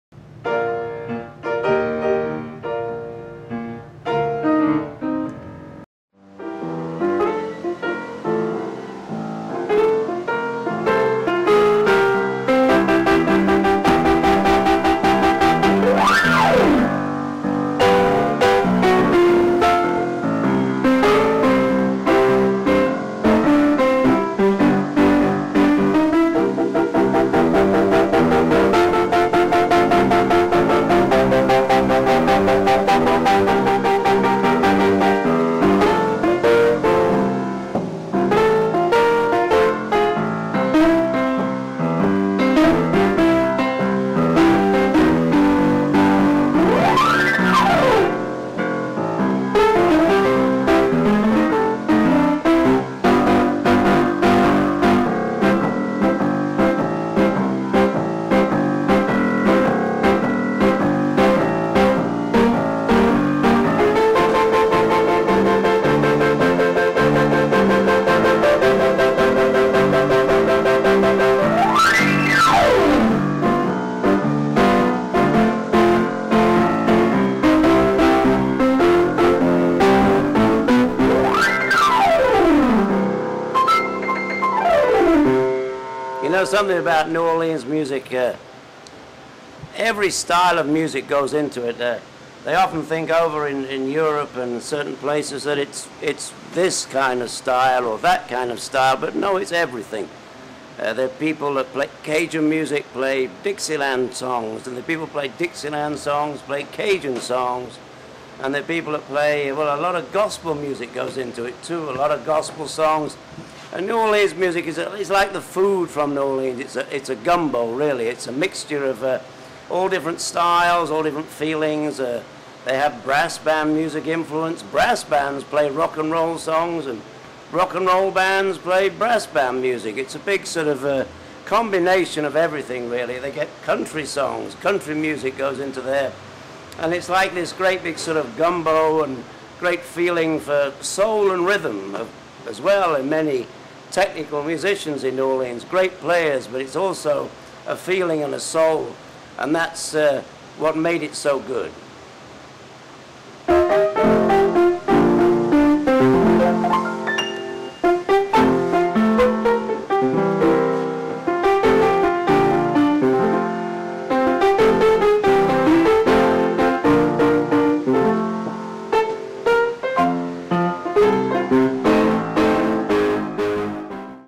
Andrew Hall's Memories of New Orleans - Preservation Hall
Interviewed by Bill Yeager
Film Cameraman - Hubie Vigreux